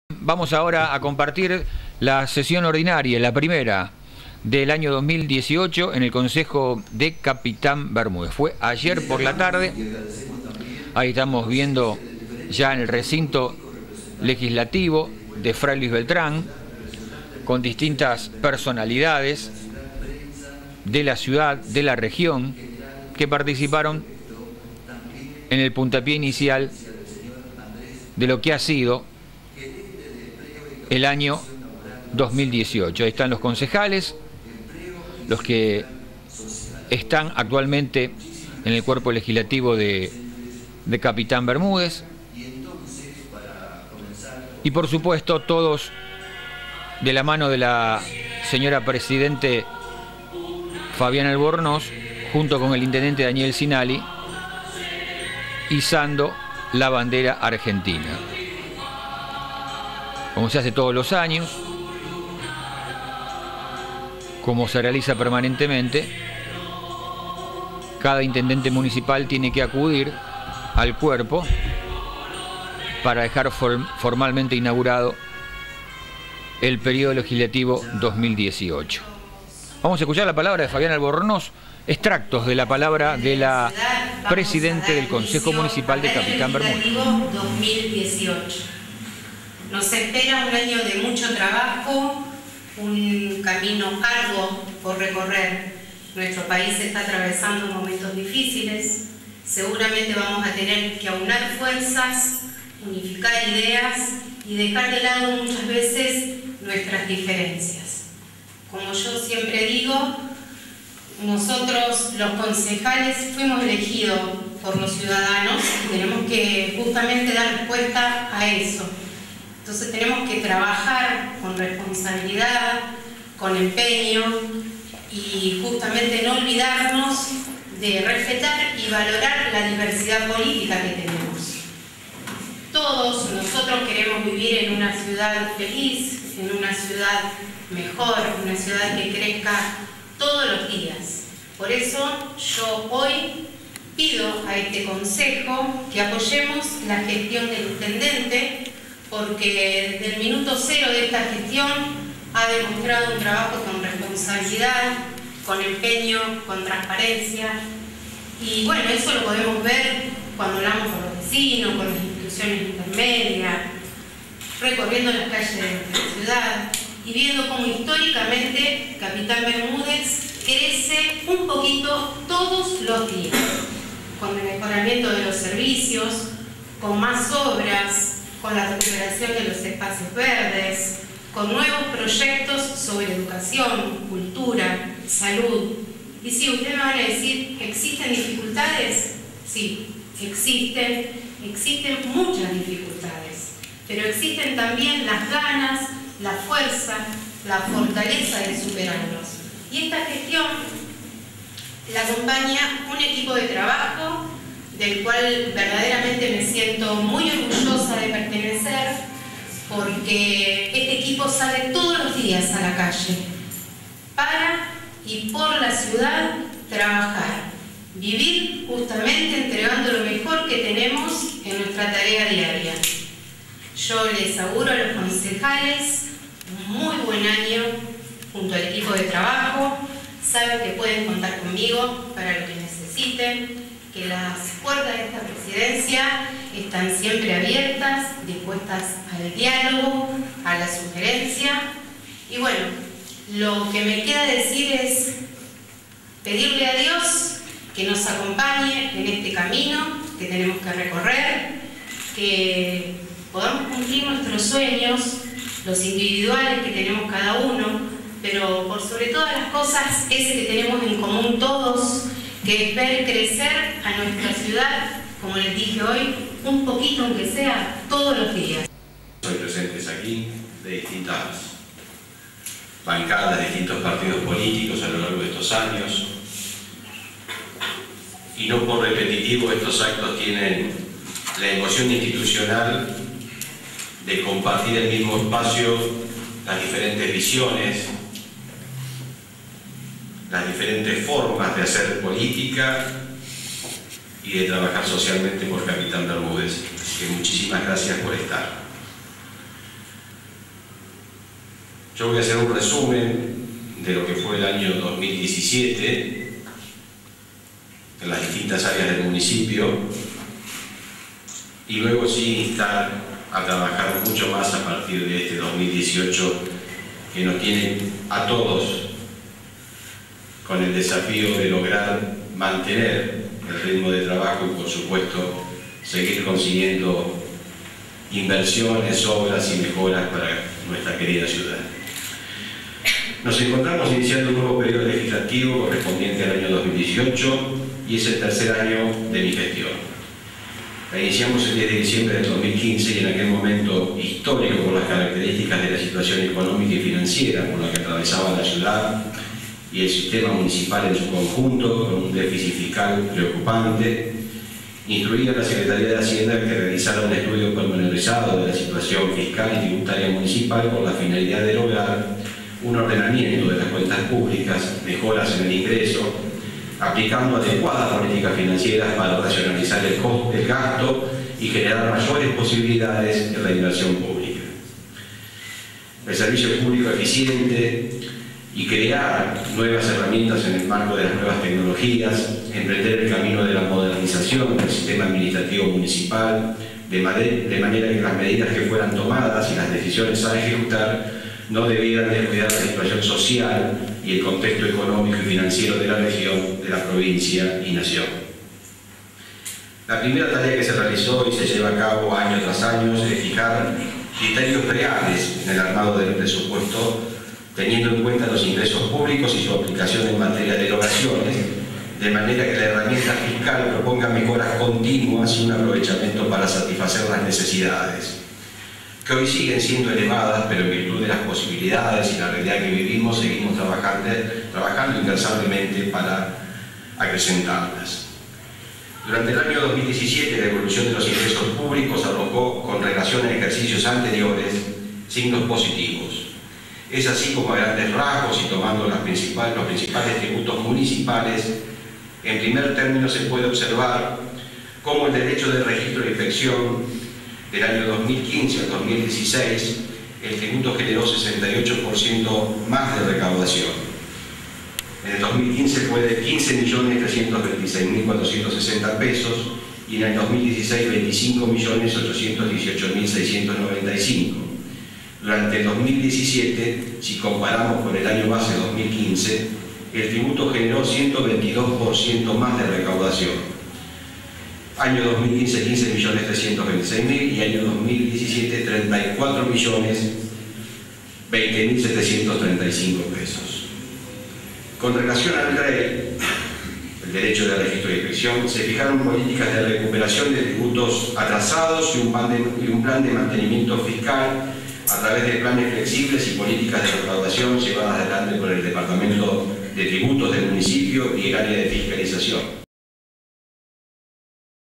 COMENZARON LAS SESIONES ORDINARIAS EN EL CONCEJO DE CAPITAN BERMUDEZ
FABIANA ALBORNOZ, PRESIDENTE DEL CONCEJO.
DANIEL CINALLI, INTENDENTE DE LA CIUDAD.